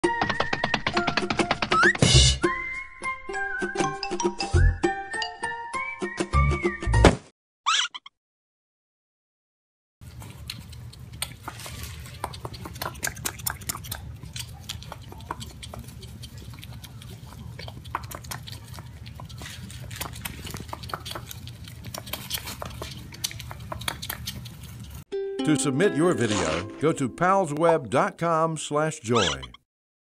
Pal's 15 Seconds of Joy "Piggin' Out on Some Pals"
Life is full of little moments of joy! Share yours with us for a chance to win an exclusive Pal's T-shirt and your 15 Seconds of Joy will be aired on WCYB's Morning News each week! Submit your video showing your 15 Seconds of Joy today